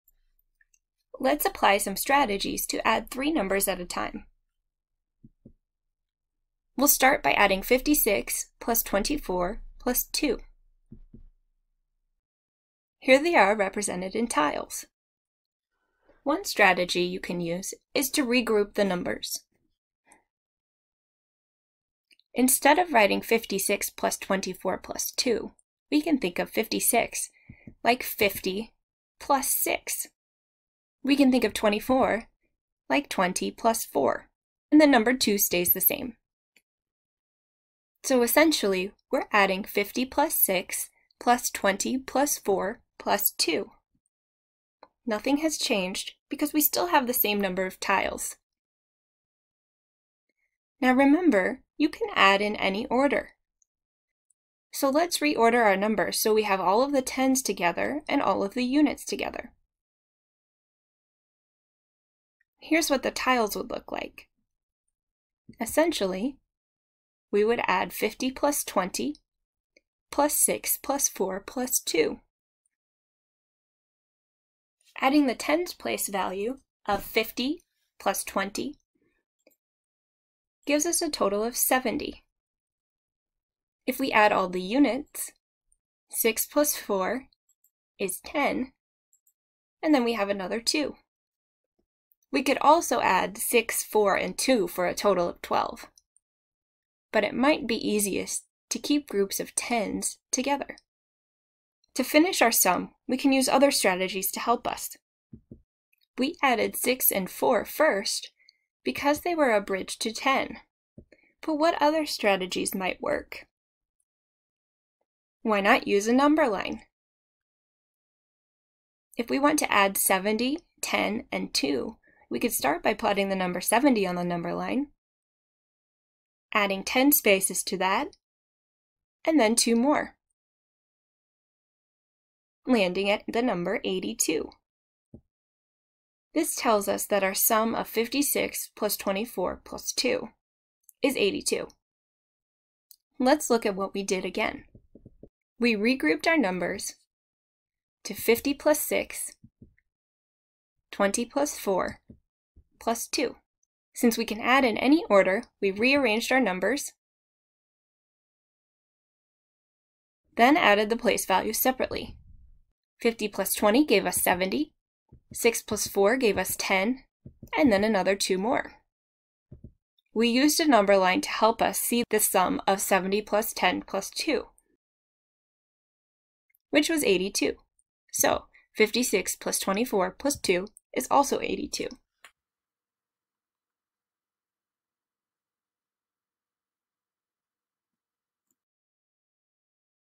Regrouping and the number line | Addition and Subtraction | Year 4
This video looks at strategies for multi-step addition when we are adding more than 2 numbers.